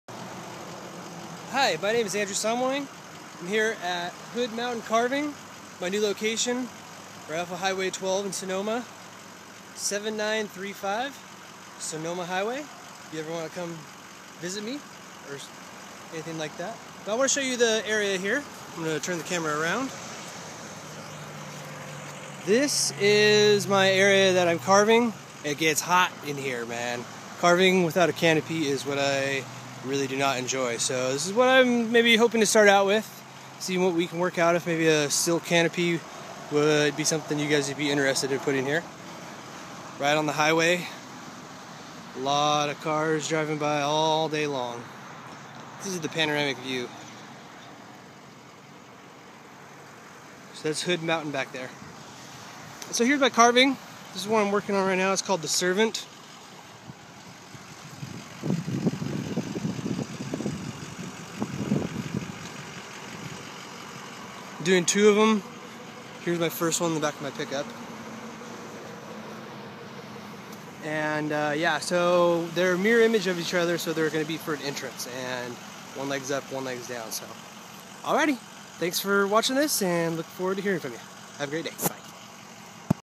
Hood Mountain Carving
This is a new chainsaw carving location called Hood Mountain Carving. My location is at 7935 Sonoma Hyw. I am looking for a sponsor that wants to start by providing a comply tent for my new location.

Thanks
Andrew Somawang